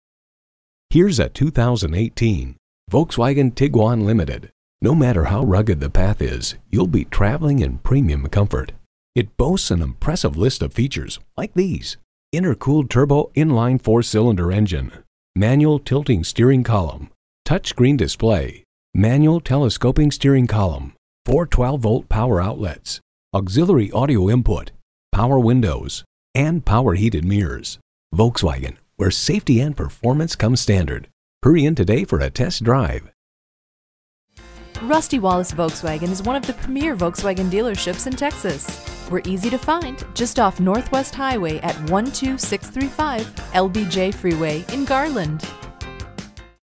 New 2018 Volkswagen Tiguan Limited Dallas TX Garland, TX V180470 - SOLD
Year: 2018
Make: Volkswagen
Model: Tiguan Limited
Trim: 2.0T
Engine: 4 Cyl - 2.0 L
Transmission: Automatic
Color: White
Stock #: V180470
VIN: WVGAV7AX8JK004390
White 2018 Volkswagen Tiguan Limited 2.0T FWD 6-Speed Automatic with Tiptronic 2.0L TSI DOHCbrbrRecent Arrival! 26/20 Highway/City MPG

Address:
12635 Lyndon B Johnson Freeway